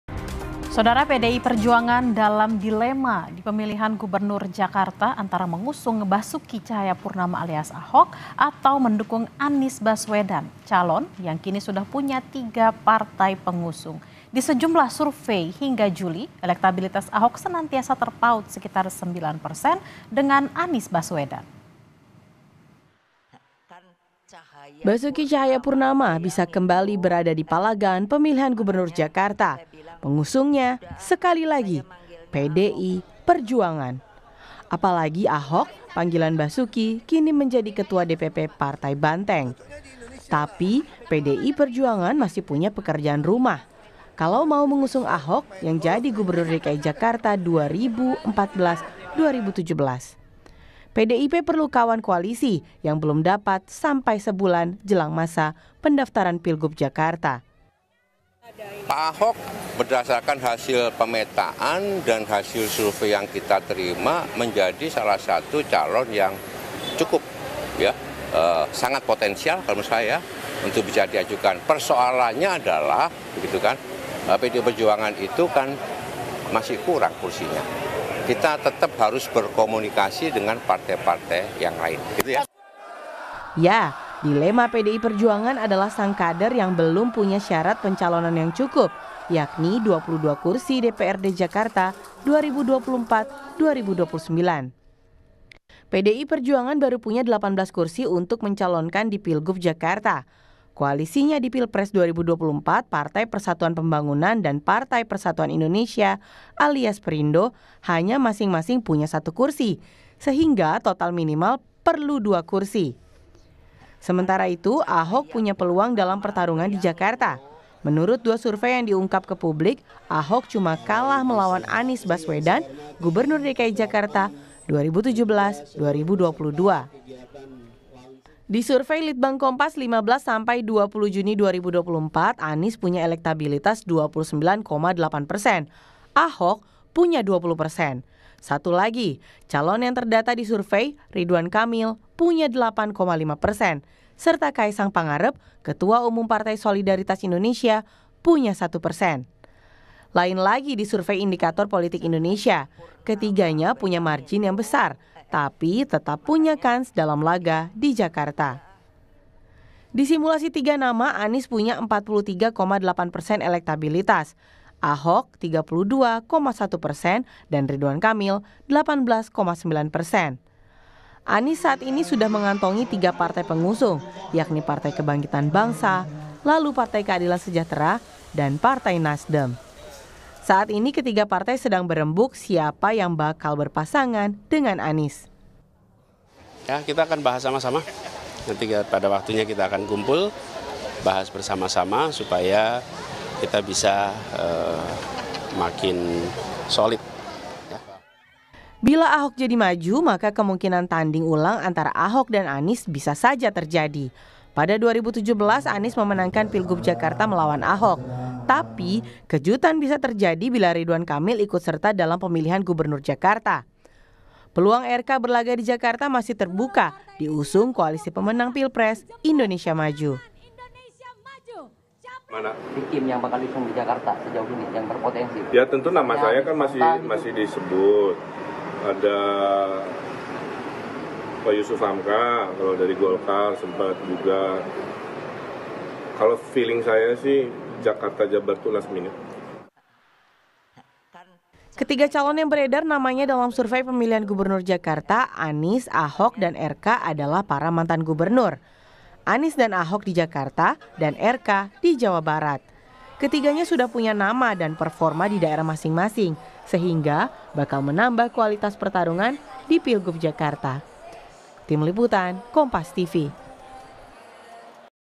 PDIP Dalam Dilema, Pilih Usung Ahok atau Anies di Jakarta?
KOMPAS.TV - Dilema PDIP di Pilgub Jakarta, Pilih Usung Ahok atau Anies Baswedan?